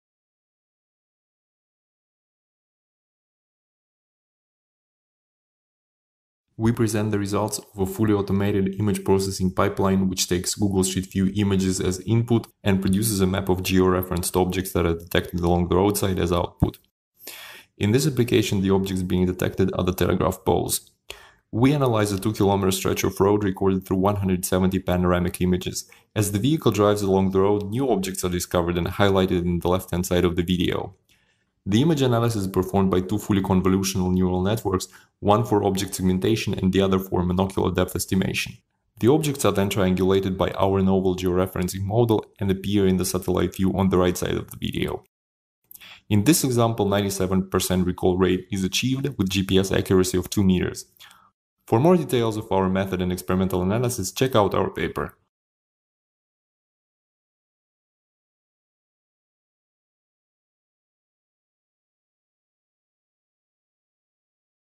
Automatic Discovery and Geotagging of Objects from Street View Imagery
Abstract: Many applications such as autonomous navigation, urban planning and asset monitoring, rely on the availability of accurate information about objects and their geolocations. In this paper we propose to automatically detect and compute the GPS coordinates of recurring stationary objects of interest using street view imagery. Our processing pipeline relies on two fully convolutional neural networks: the first segments objects in the images while the second estimates their distances from the camera. To geolocate all the detected objects coherently and merge together instances observed from multiple images we propose a novel Markov Random Field model based on triangulation. We show experimentally the effectiveness of our approach on two object classes: traffic lights and telegraph poles. The experiments report high object recall rates and GPS accuracy within 2 meters, which is comparable with the precision of single-frequency GPS receivers.